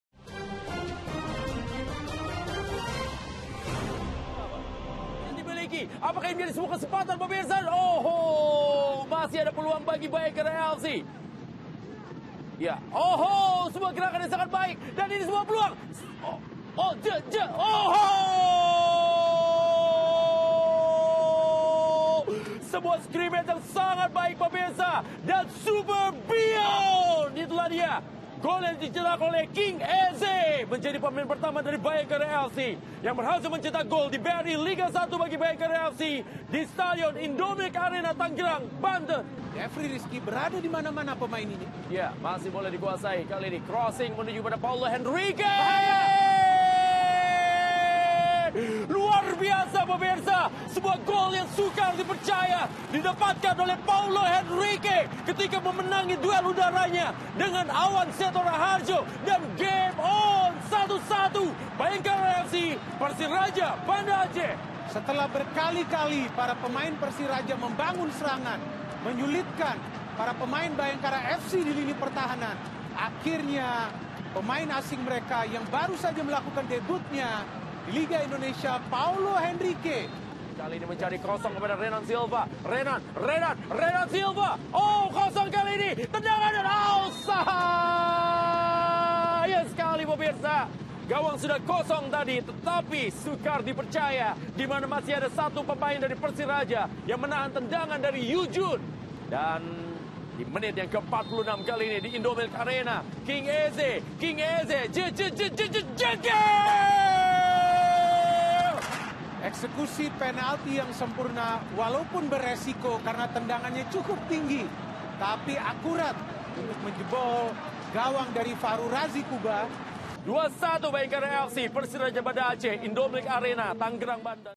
Paulo Henrique Cetak Gol Debut di Liga 1 - Bhayangkara Fc v Persiraja
Laskar Rencong TV - Persiraja Pedia

Bagaimana komentar Anda?